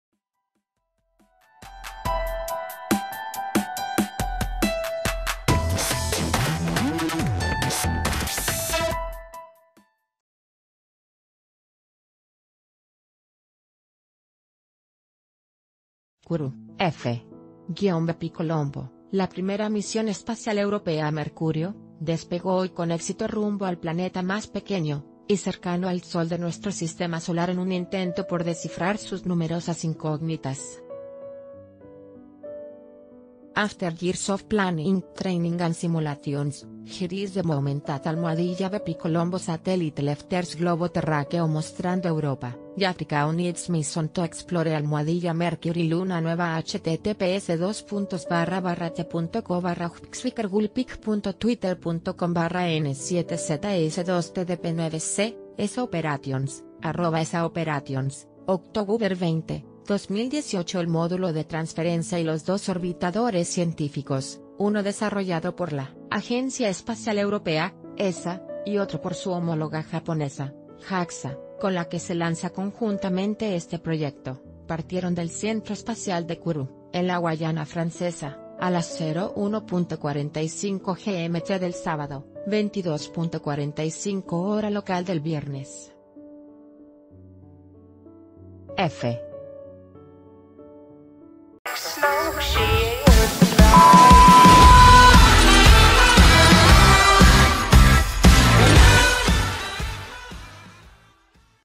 Despega con éxito misión espacial a Mercurio
Kurú (EFE).- BepiColombo, la primera misión espacial europea a Mercurio, despegó hoy con éxito rumbo al planeta más pequeño y cercano al Sol de nuestro Sistema Solar en un intento por descifrar sus numerosas incógnitas. After years of planning, training and simulations, here is the moment that Bepi...